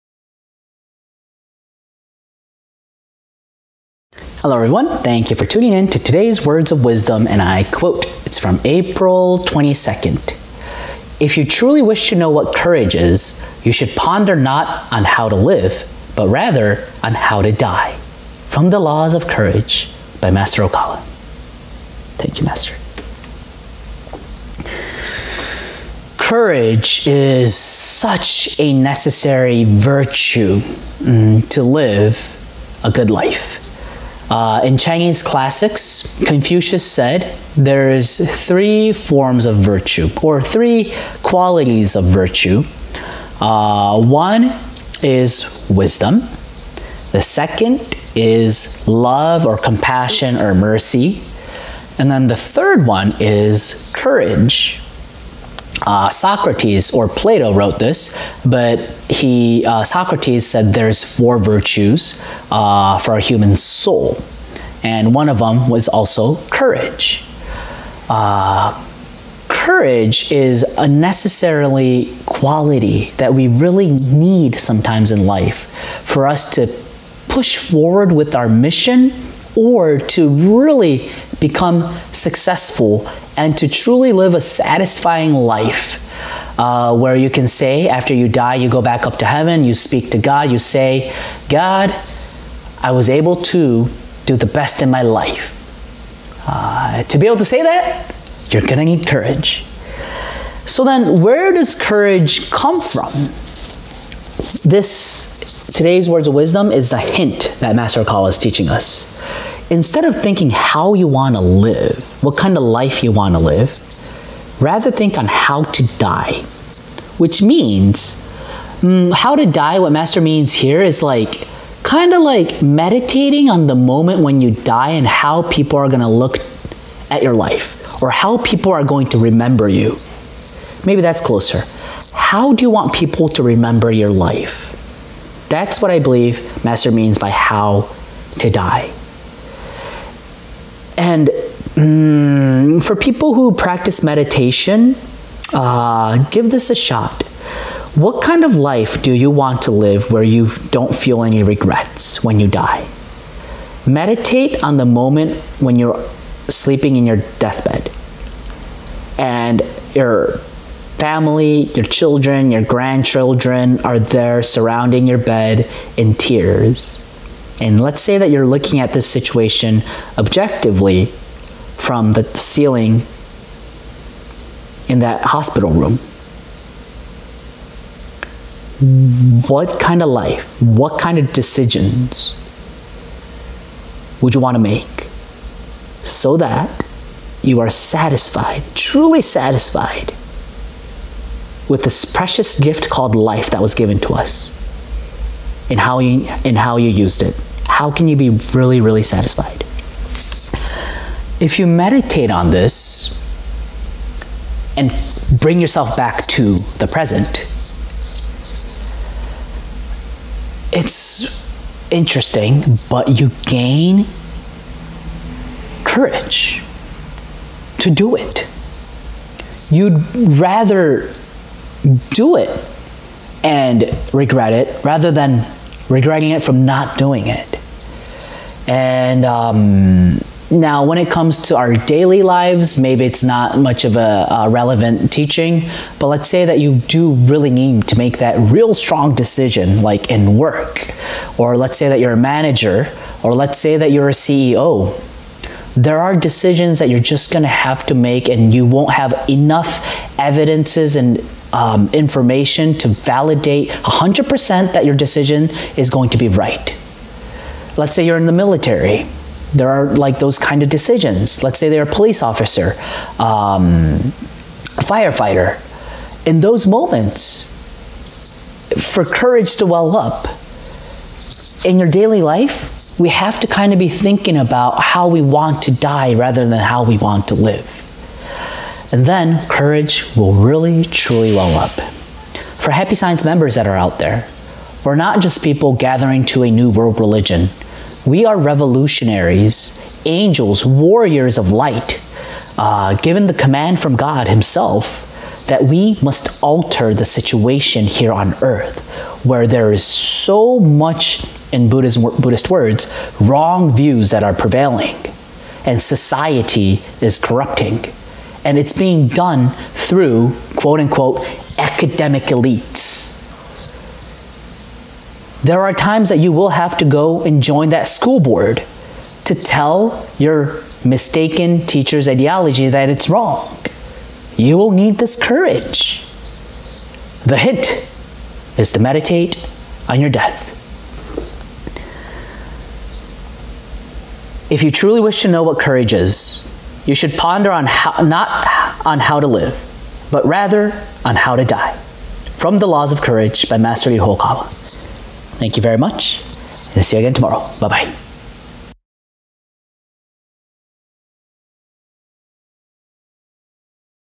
Words of Wisdom - If you truly wish to know what courage is, you should ponder not on "how to live"
✨ If you truly wish to know what courage is, you should ponder not on "how to live" but rather on "how to die."

📚From "The Laws of Courage" written by Ryuho Okawa